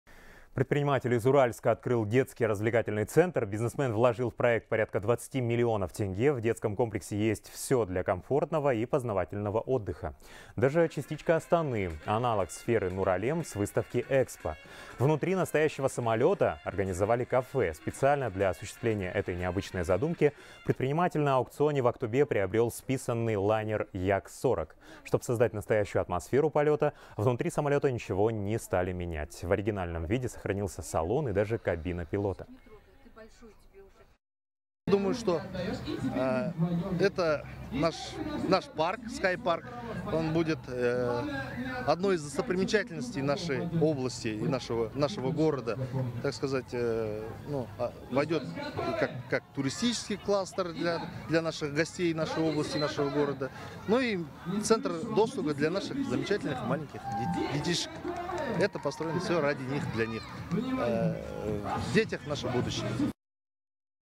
Бизнесмен из Уральска вложил 20 млн тенге в детский развлекательный центр
Предприниматель из Уральска открыл детский развлекательный центр. Бизнесмен вложил в проект порядка 20 миллионов тенге.